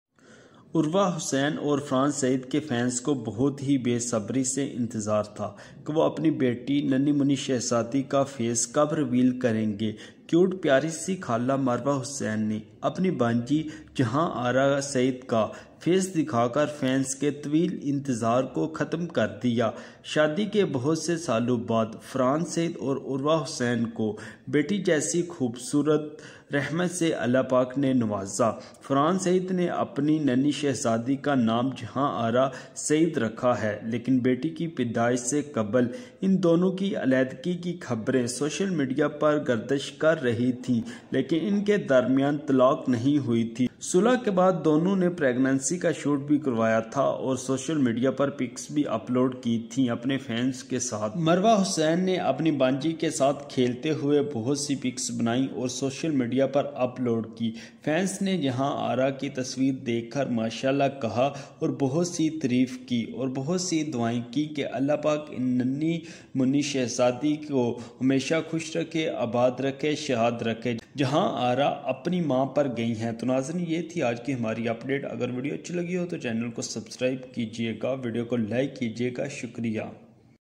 Marwa Hussain Revealed Farhan Saeed Daughter Face | Urwa Hocane Daughter Face Revealed
Asslam o alaikum ¦ Aj ki es Video mein  me apko Famous Actrees ki Beti ki Birth k baray mein batao ga,

Farhan saeed  k ghar nani payri se beti ki pedaaash hoe hai,
urwa bohat he khush hai,
Allah pak enki beti ko salamat rakhaye ameen😍